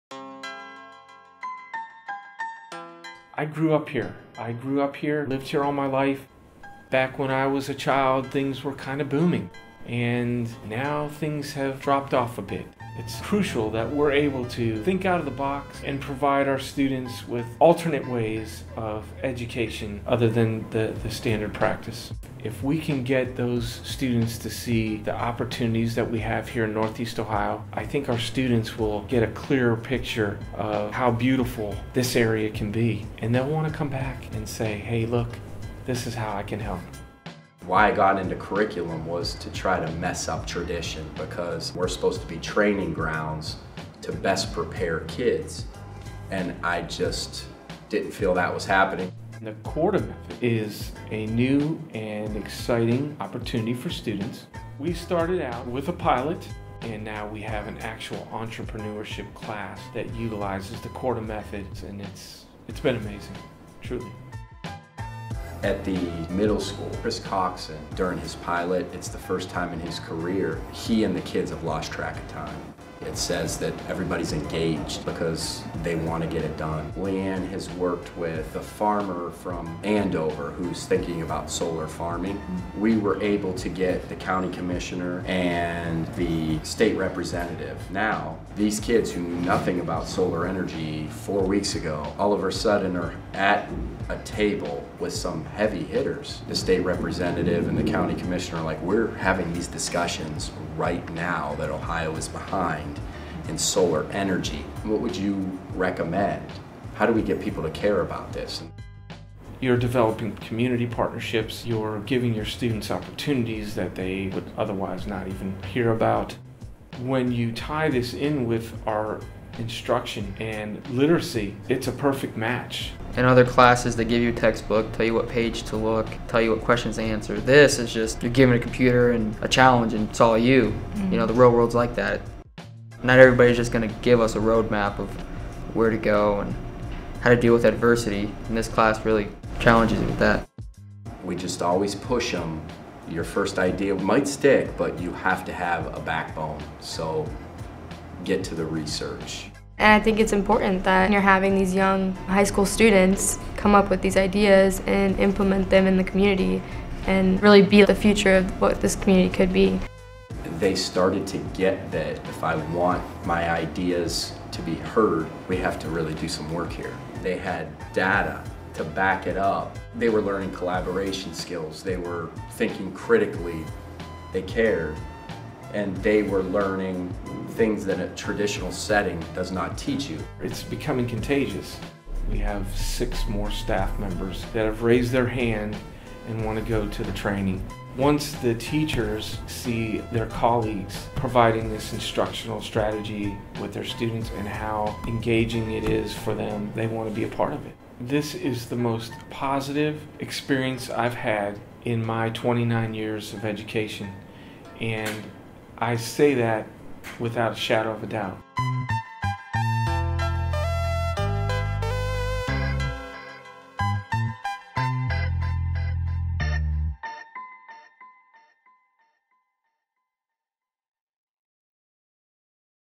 Buckeye Local Schools Spotlight
See what happens when a rural public school district decides to change the future for their students..and their community. Buckeye School District leadership partners with Korda Institute for Teaching to change the way teachers teach and students learn in Ashtabula, Ohio. Educators throughout the district are using this radically different teaching method to create innovative classes where students learn while solving real and urgent challenges in their community.